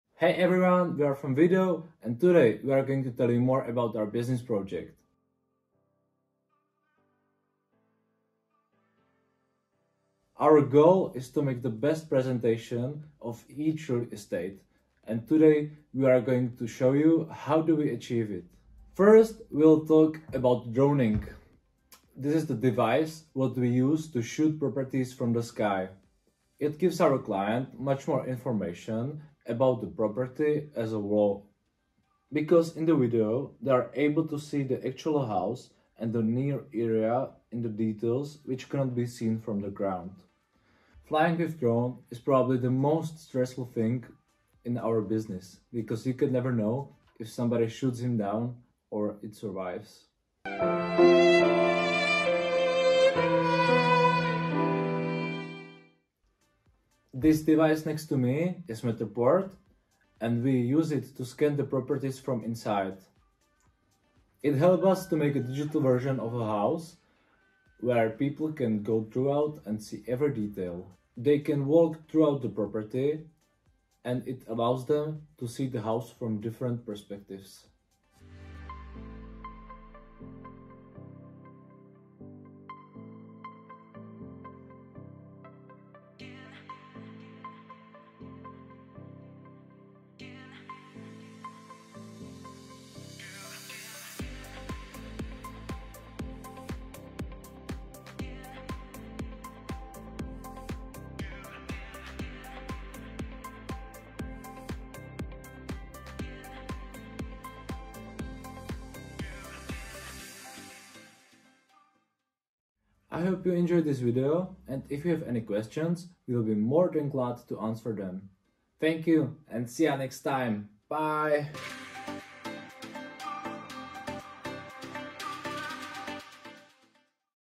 Meet the projects: V3DO
In this new series, we will introduce you to the projects, that are part of ESBD. First up, let us present you V3DO!

We are two guys from Brno Faculty of Business and Management, and we set a goal of upgrading real estate presentation to another level.

Watch the video to get a better idea of what we do. 📹🏘

If you'd like to get in touch don't hesitate to DM us 👉 @v3do_cz @jakub_schmidt @zrzavey_ 👋